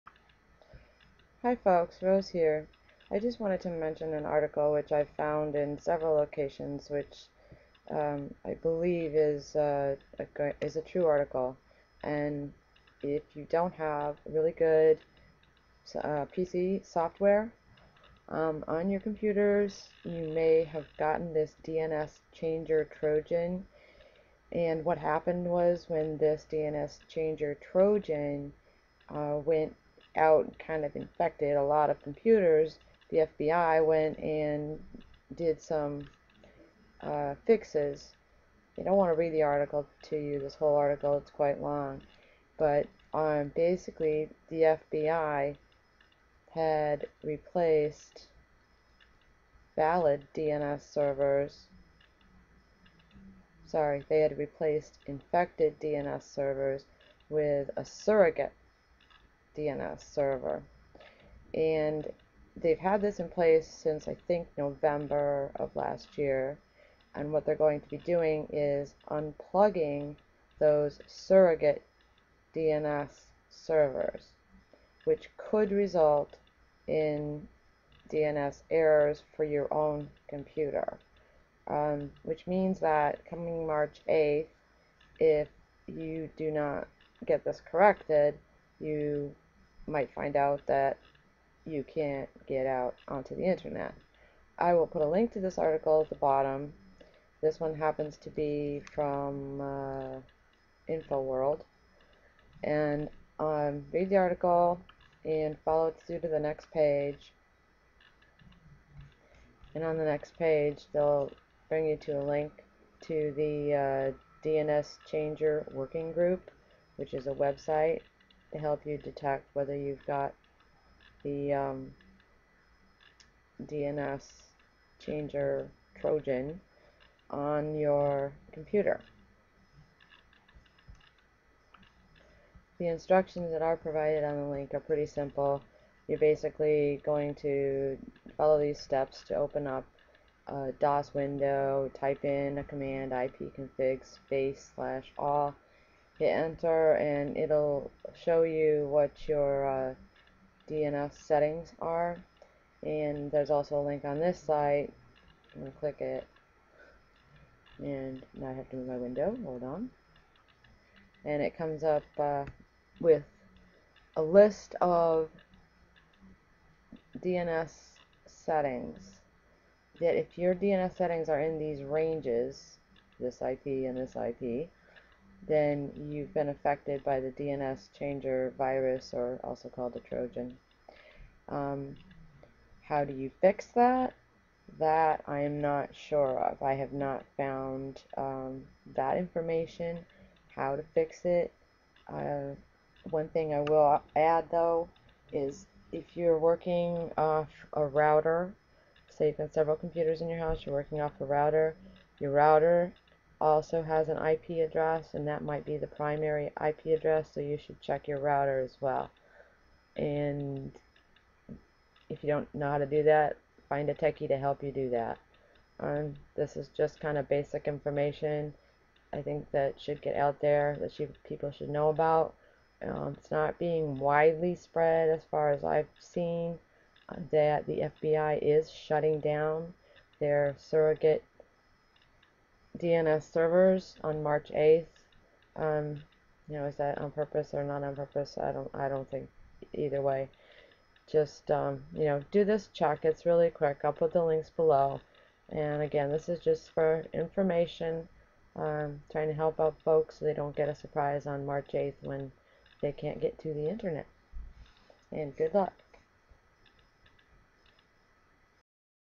Check for DNSChanger Trojan BEFORE March 7, 2012- FBI "fix" unplug date-Please share!
"Companies and home users whose computers or routers are infected by the DNSChanger Trojan risk being unable to access the Web come March 8, 2012, when the FBI unplugs the legitimate DNS servers it set up to replace the rogue DNS servers that were forwarding victims to malicious sites. The removal of the fed's band aid could impact a substantial number of users, too, as half of Fortune 500 companies and government agencies are infected with the malware, according to a new report." (Quote from the source article listed below:)

(Note: Good PC security software should already have caught this, but if you don't have it, get it. "Malwarebytes" had a free scan  available at one time, not sure if it is still available or free)